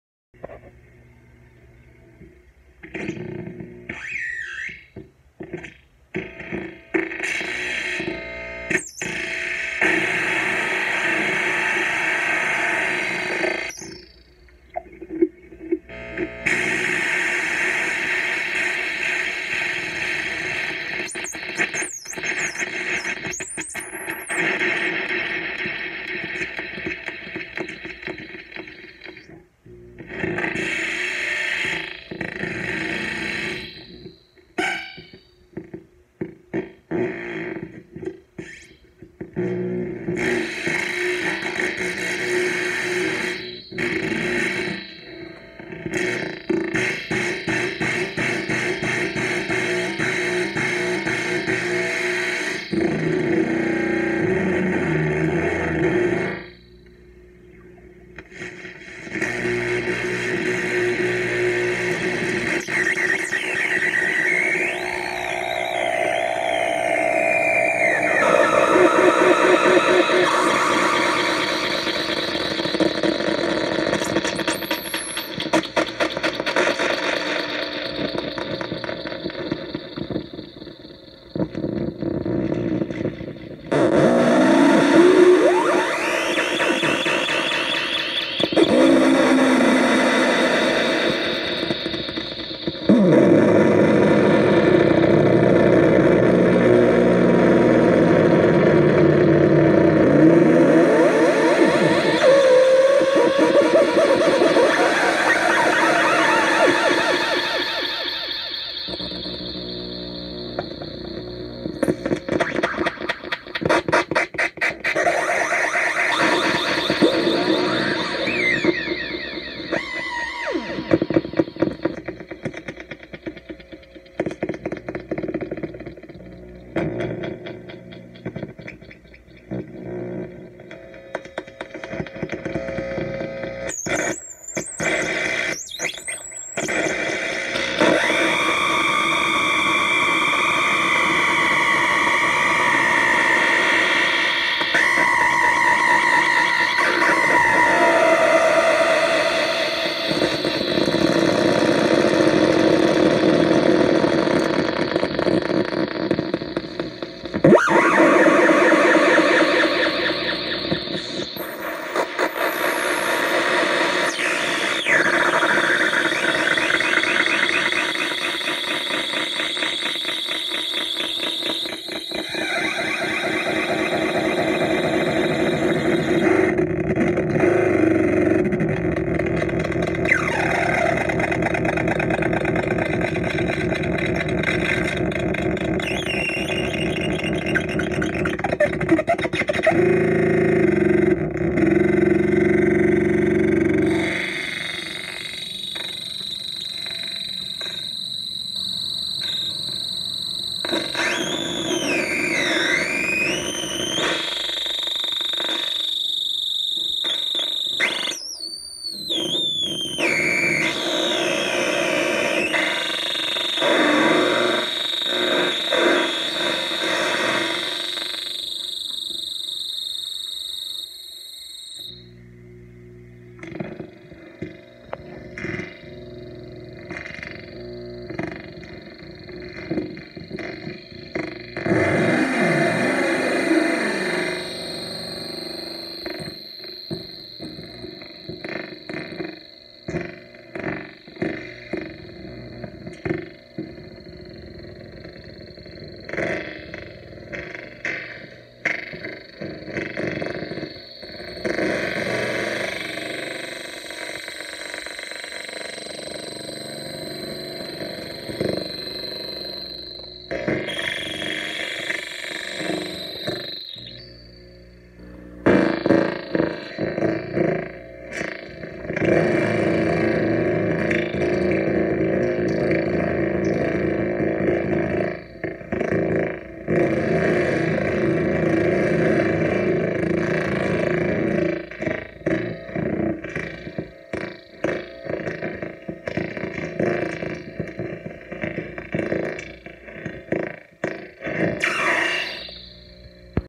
Clip 2 Phantom Temple
Sound meditation device. The Psychic Temple Transmitter. Experimental noise machine with built in echo/delay. It’s like circuit bending with your fingers. Tons of static and squealing sounds has been known to pic up radio signals.